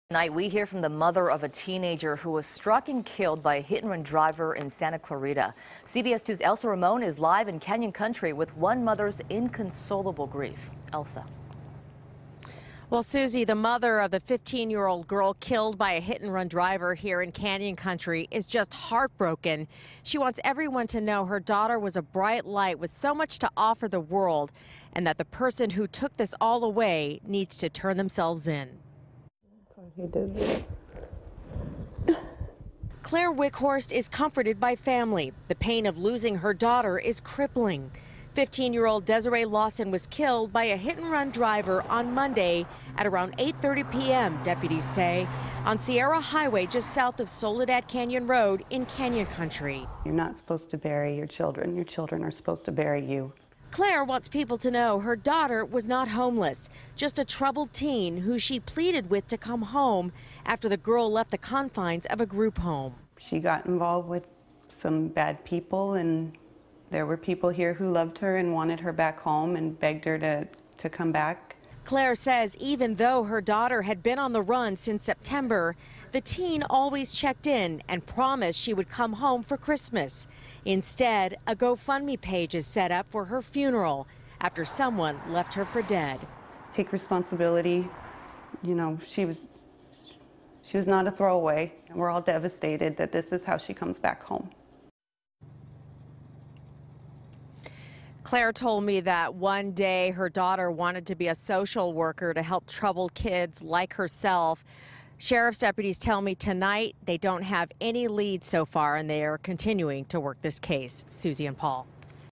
Grieving Mom Recalls The Teen Daughter Killed In Hit-And-Run Accident
Claire Wickhorst is hoping the driver who killed her 15-year-old daughter will come forward. Elsa Ramon reports.